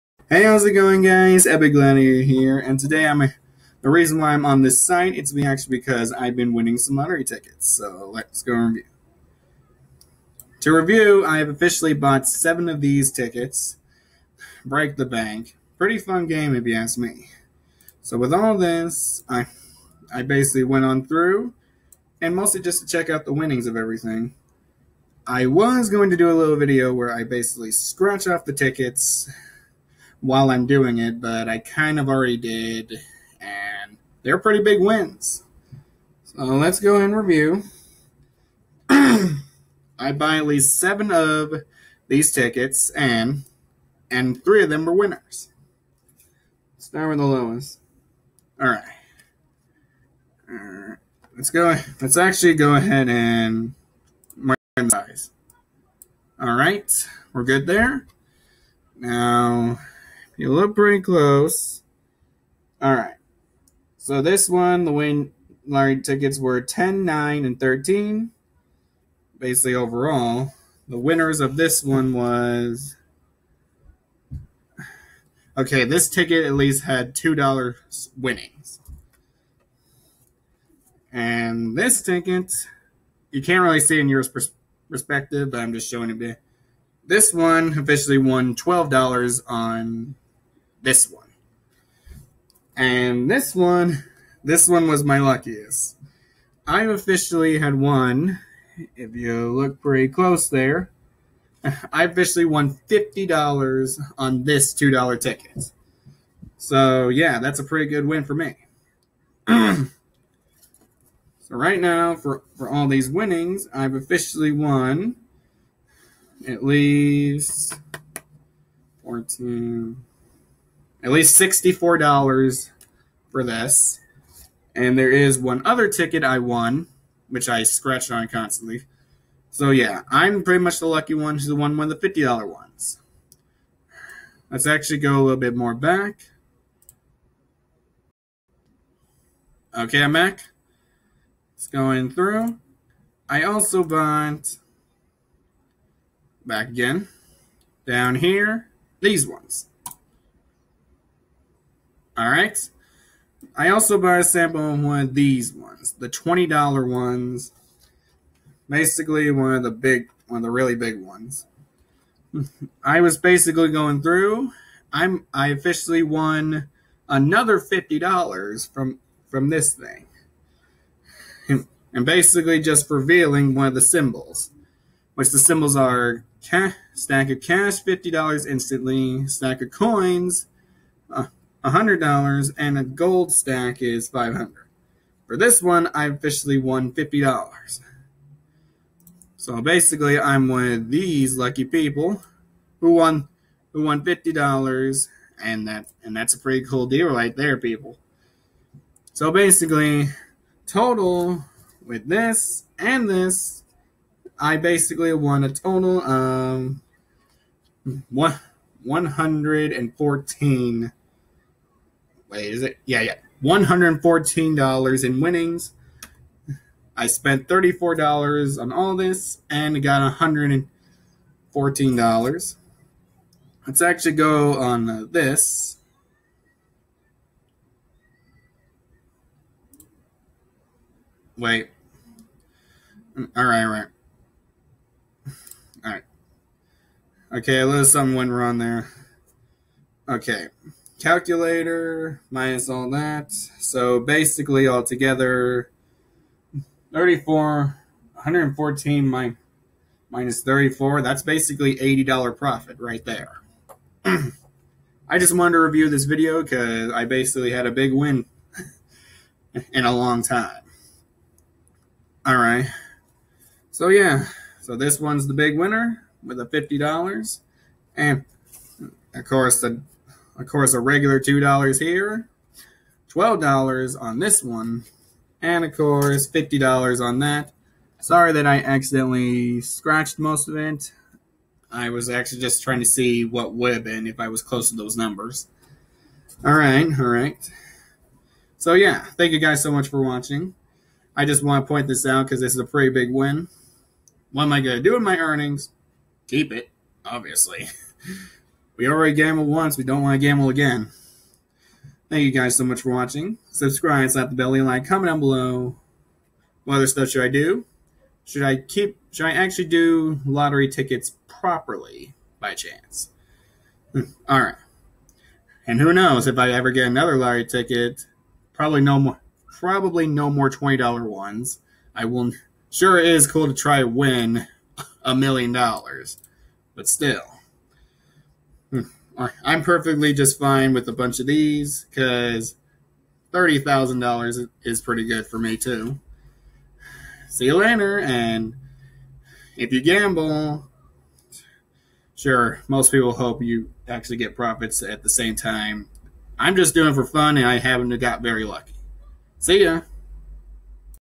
LOTTERY WINNER- Spent $34 on Lottery Tickets, won $114
Break the bank $2
- $2
- $12
- $50
MONEY $20
- $50